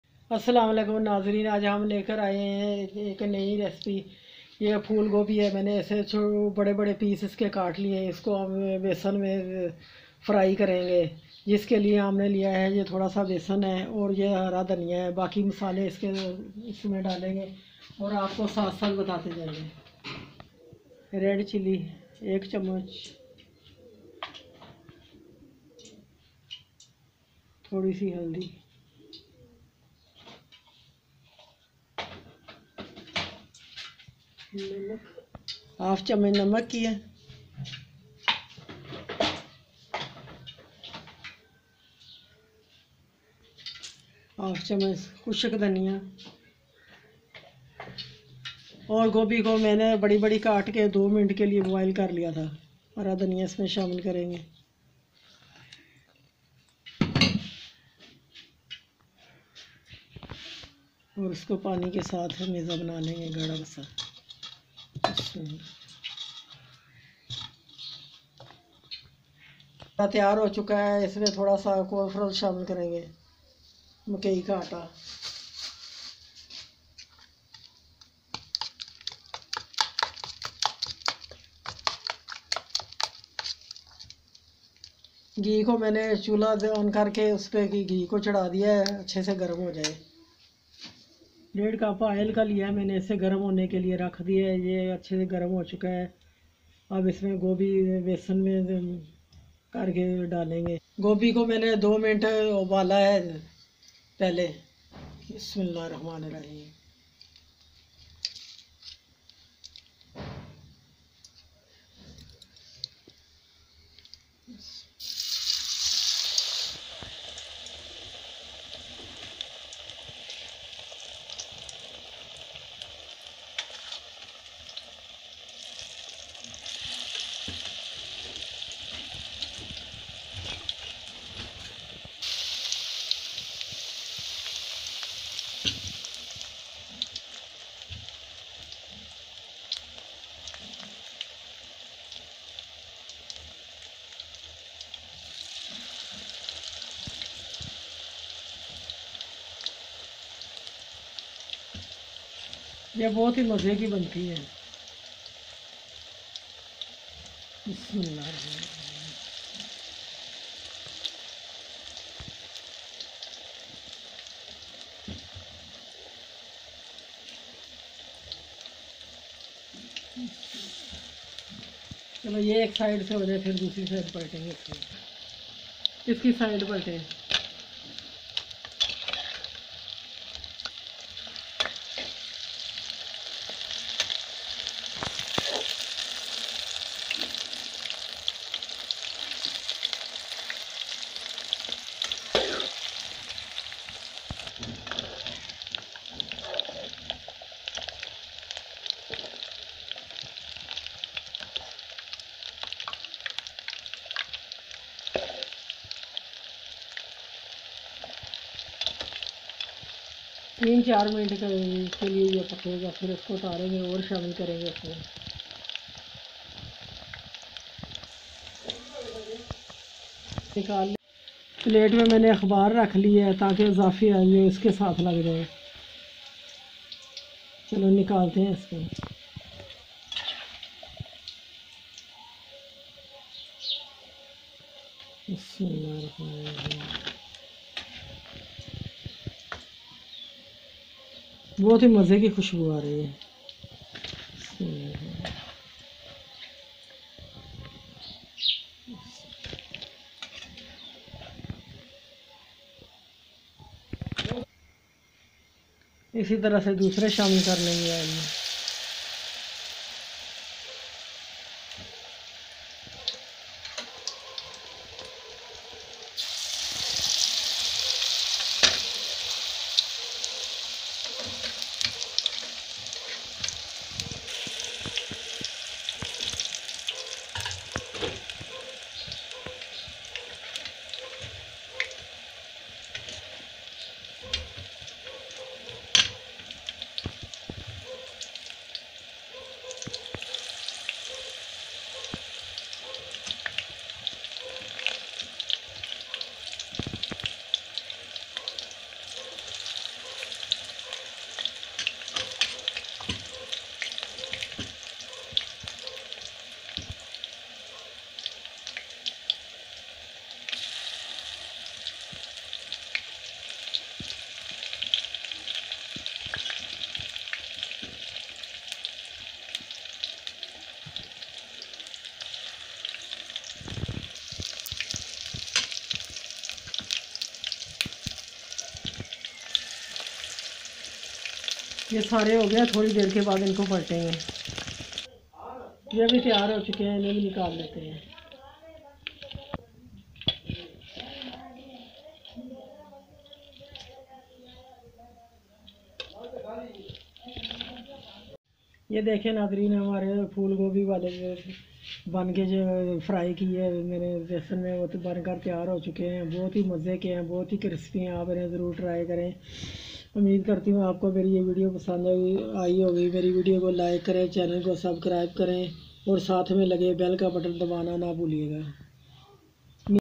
Crispy Gobhi Fry || Cauliflower Fry Tasty Recipe
Aj ki Video Mai Hum Apko Bnana Sikha Rhn Hain Crispy Gobhi Fry Asaan Triky Se.

Another Recipe Link Here
👉Turnip Spinach Sabzi👇